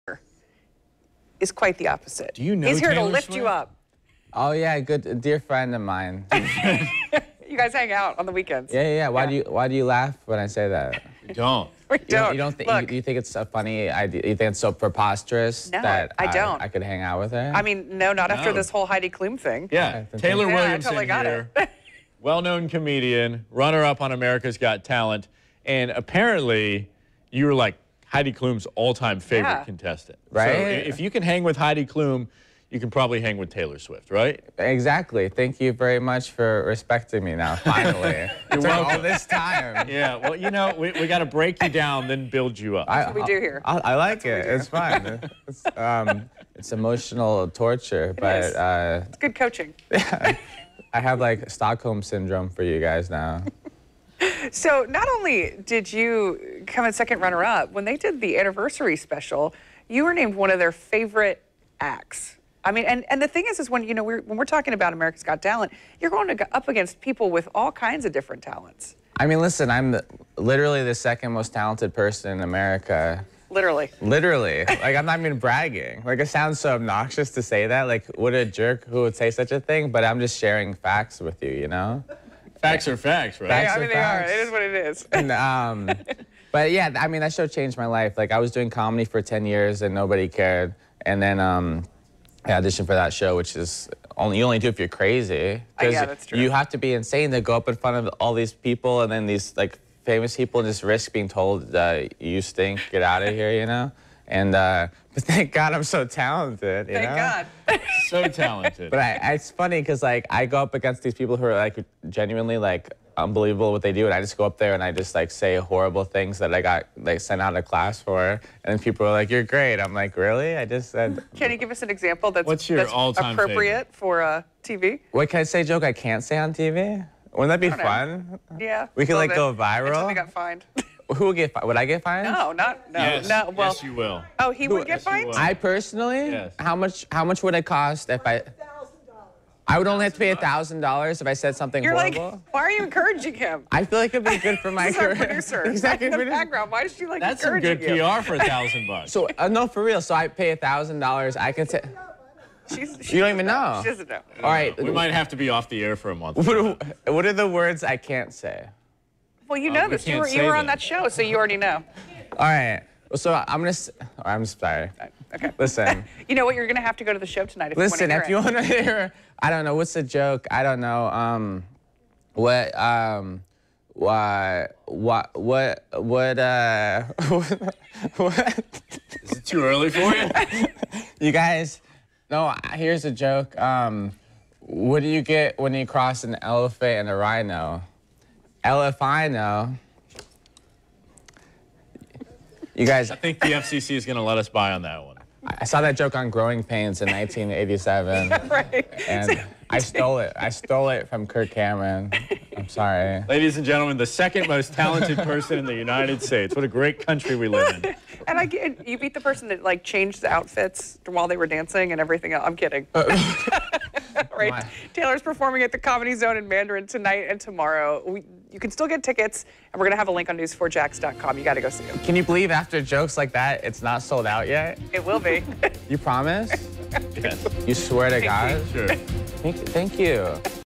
Taylor Williamson at the Comedy Zone
Comedian, Taylor Williamson, joins us on the Morning Show before performing at the Comedy Zone.